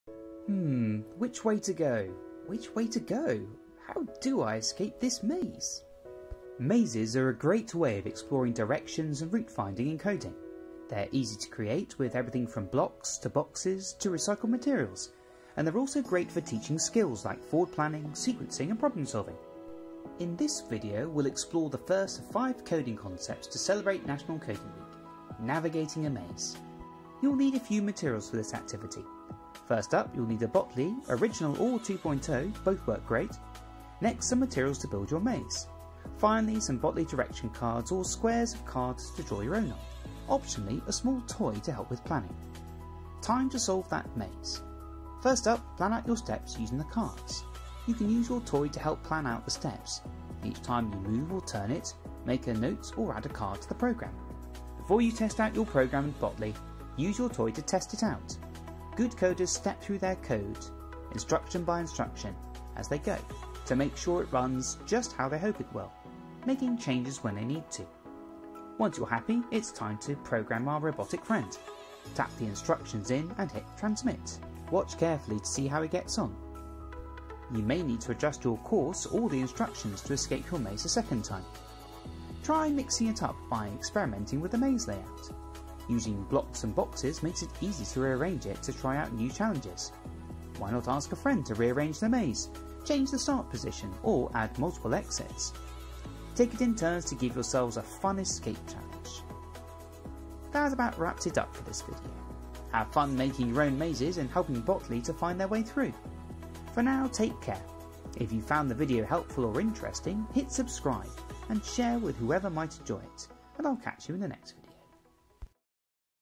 Botley Coding #1 Navigating a maze
In celebration of national coding week, here's the first of three videos using Botley to explore coding fundamentals and projects.

In this first video, we explore how to navigate a maze using Botley. We'll look at path planning, sequencing and touch on path optimisation.

In the rest of the series, we'll explore object avoidance and fetching objects!